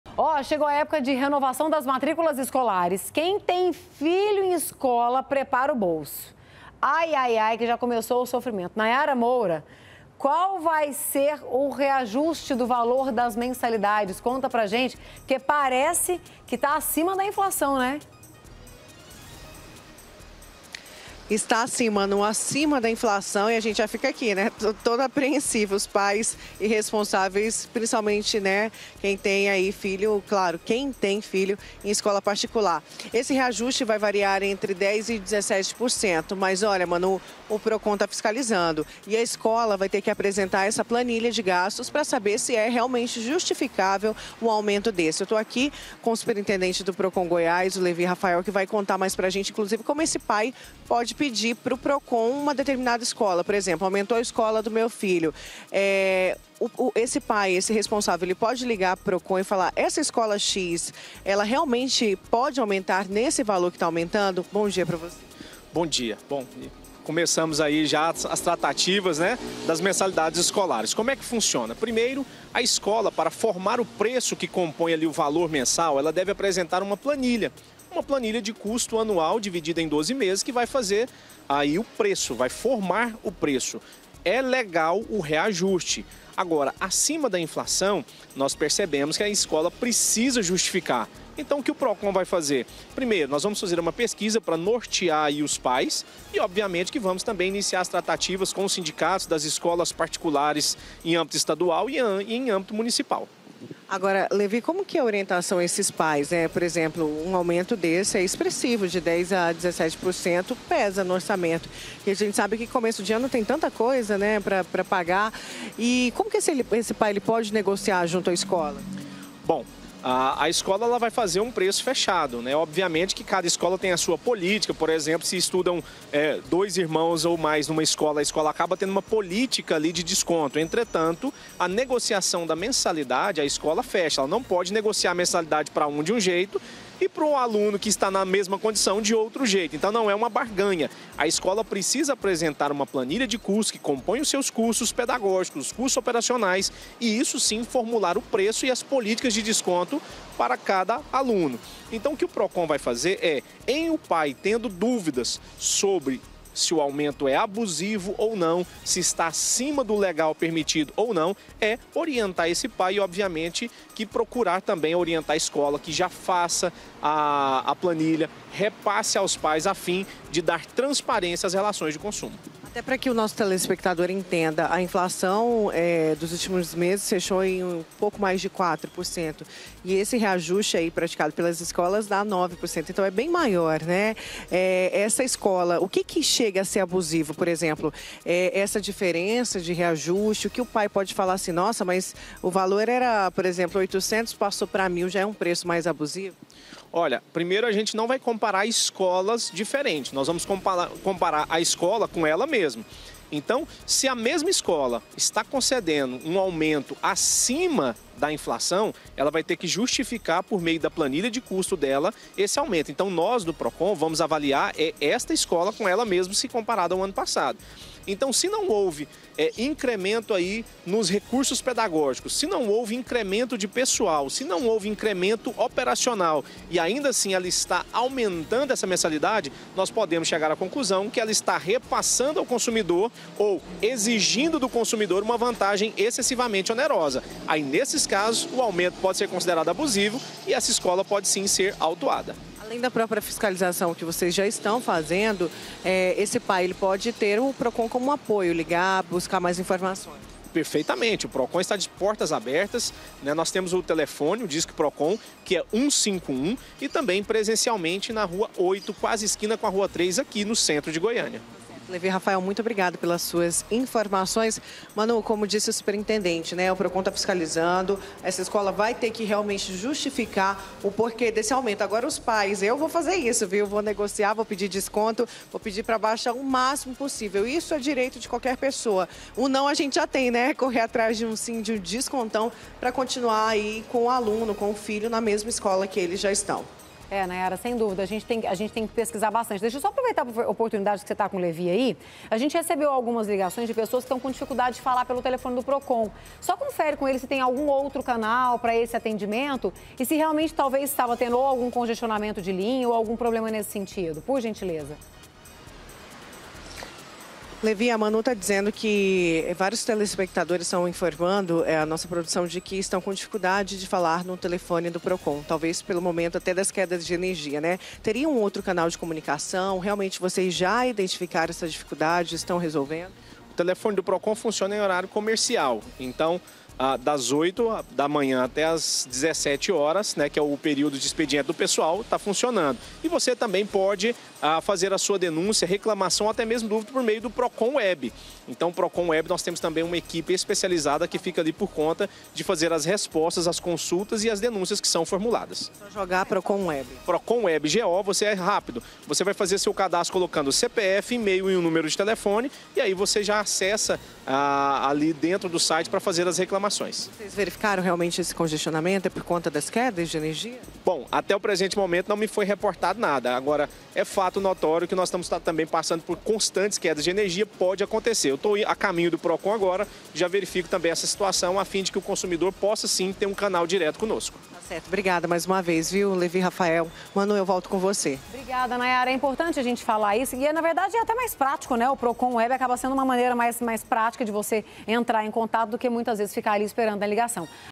MENSALIDADE ESCOLAR: AUMENTO VAI SER ACIMA DA INFLAÇÃO
CHEGOU A ÉPOCA DE RENOVAÇÃO DAS MATRÍCULAS ESCOLARES E QUEM TEM FILHO NA ESCOLA, PODE PREPARAR O BOLSO.